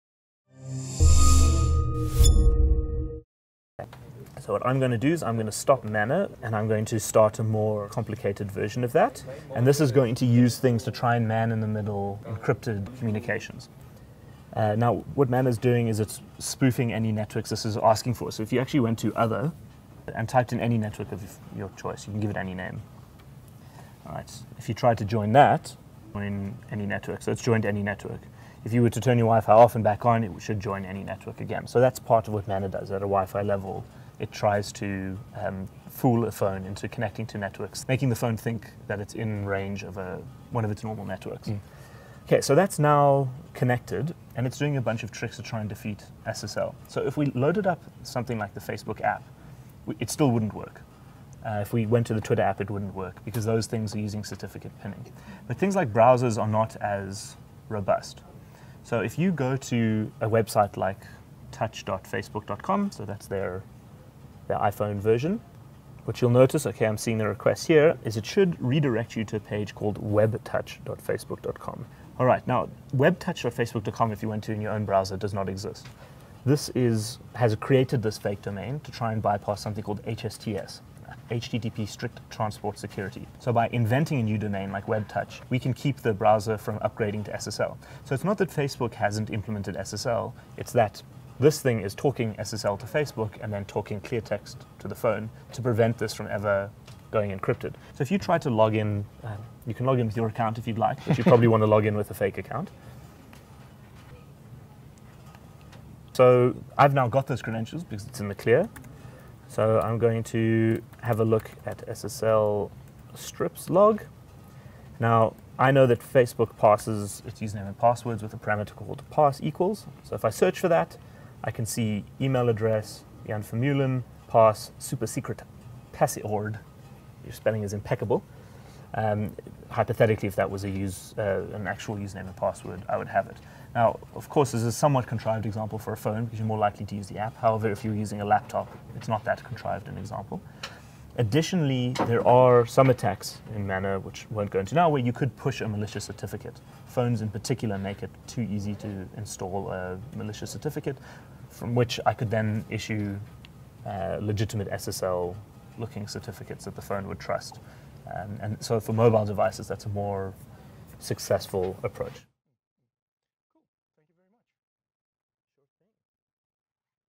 W-Fi hacking: working around SSL encryption (man-in-the-middle)
SensePost CTO Dominic White shows how hackers might circumvent SSL encryption and get your username and password.

For a write-up of the demo, see the article on MyBroadband